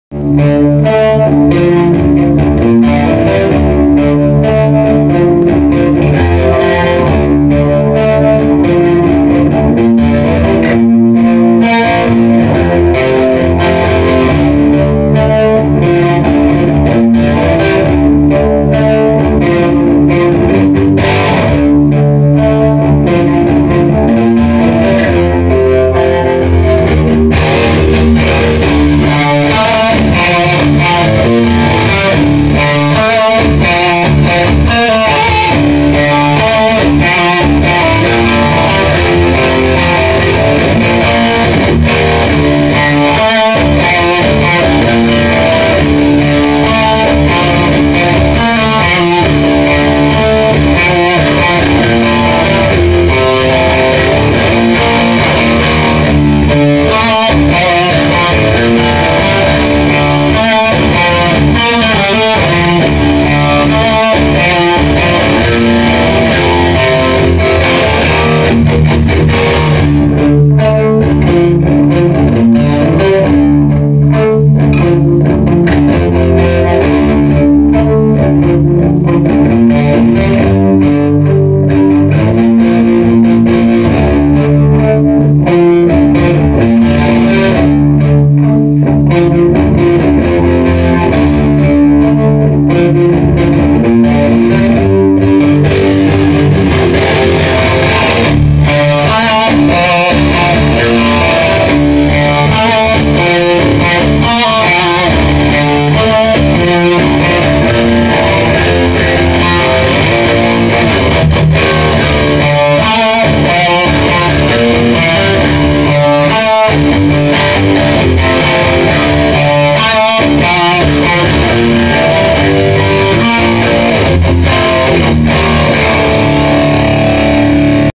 New pedal dayz jam
Lil jam with the new Silencer and Sea Machine. And the volume pedal cause it's life.